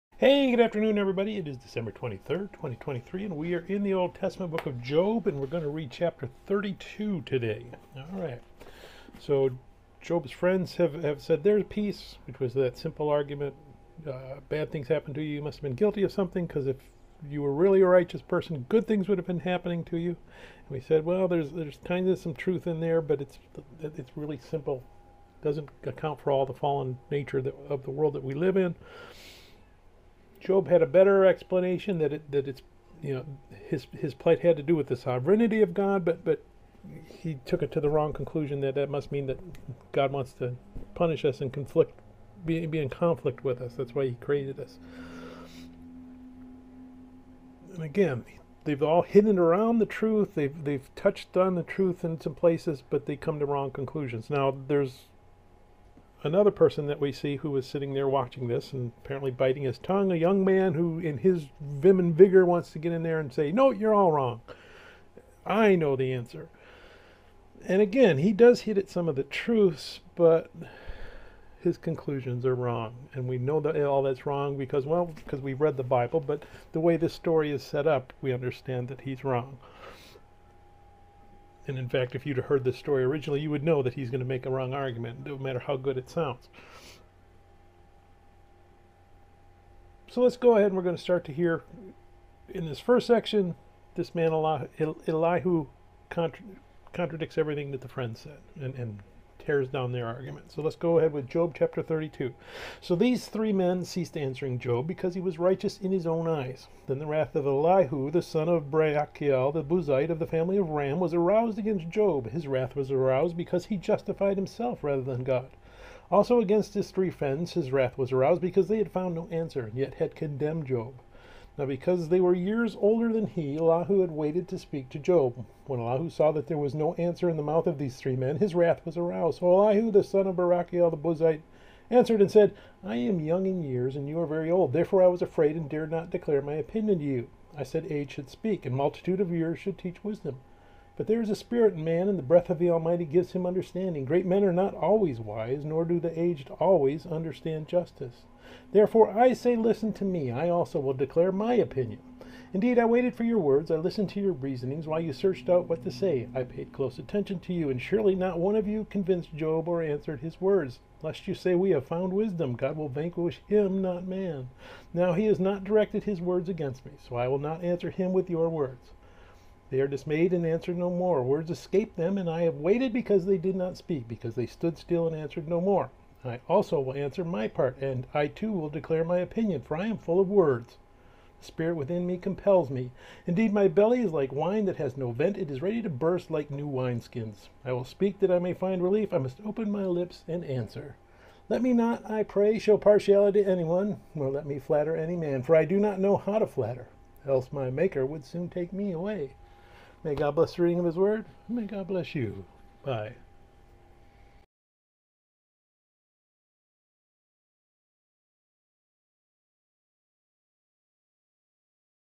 Day 357, Job 32 NKJV
Helping you keep your New Year's resolution to read your Bible, by doing the Pentateuch and Isreal History reading with you.